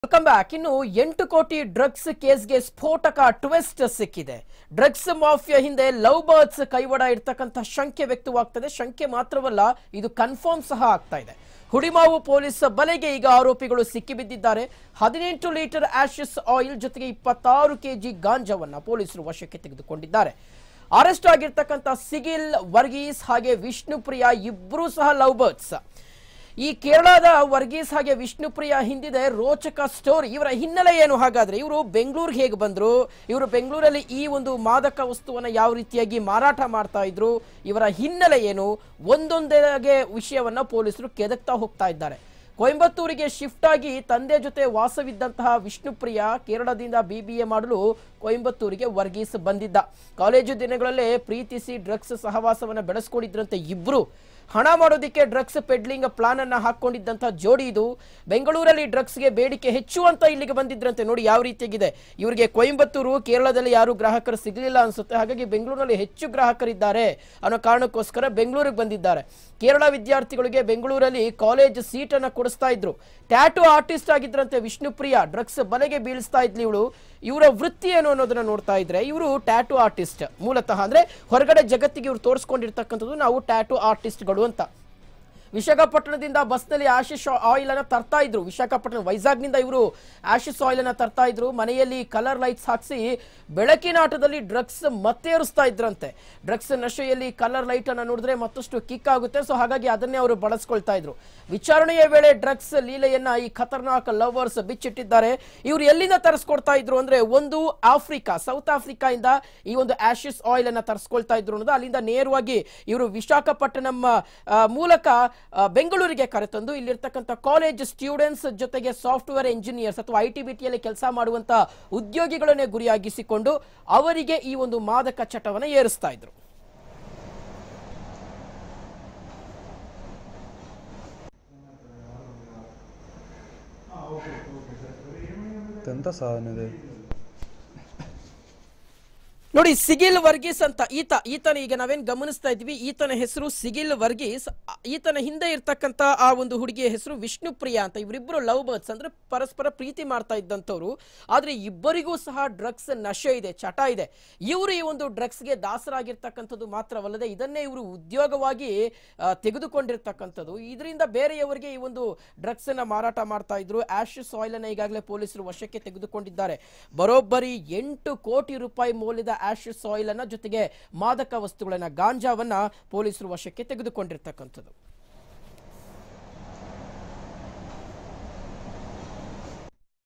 8 ಕೋಟಿ ಮಾದಕ ವಸ್ತುವಿನ ಹಿಂದೆ ಲವ್​​​ ಬರ್ಡ್ಸ್​ ಕೈವಾಡ! LOVE_BIRDS
Btv News Kannada:8 ಕೋಟಿ ಮಾದಕ ವಸ್ತುವಿನ ಹಿಂದೆ ಲವ್​​​ ಬರ್ಡ್ಸ್​ ಕೈವಾಡ.. ಹುಳಿಮಾವು ಪೊಲೀಸ್ ಬಲೆಗೆ ಬಿದ್ದಿರೋ ಆರೋಪಿಗಳು! LOVE_BIRDS |